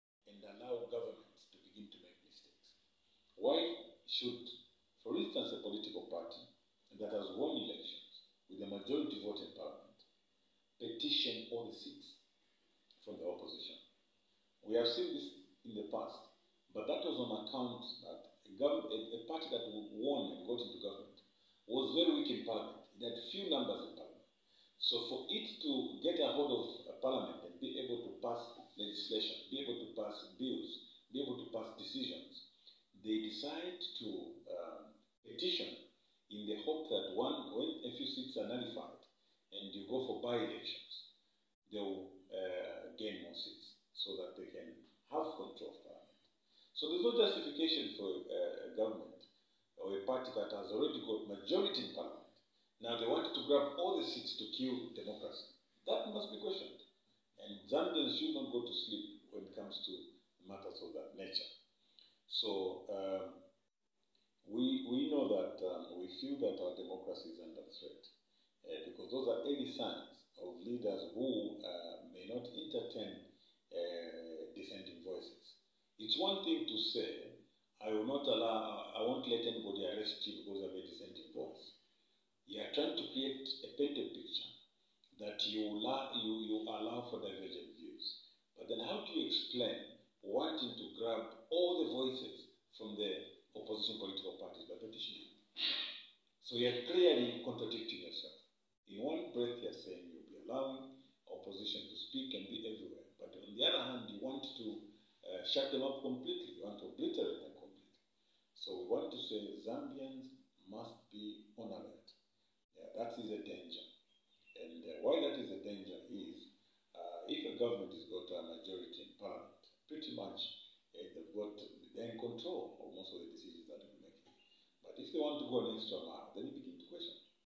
Mundubile Accuses UPND Government Of Attempting To Turn Zambia Into Single Party State
Leader of opposition in parliament Brian Mundubile has alleged that it is possible the President Hakainde Hichilema and his government will try by all means and turn Zambia into a one-party state going by what has so far been exhibited within a few days it has been in office.
Mr. Mundubile who is also Mporokoso member of parliament says the UPND’s behaviour towards the selection of speakers of the national assembly on Friday was a clear signal that Zambia is headed for the worst stating that this is despite the sweet talk by Mr. Hichilema about inclusivity.
He has also cited the parliamentary petitions from the UPND losing candidates who have decided to petition almost all the seats won by PF candidates and other independents as one of the ways it intends to silence the opposition in the country.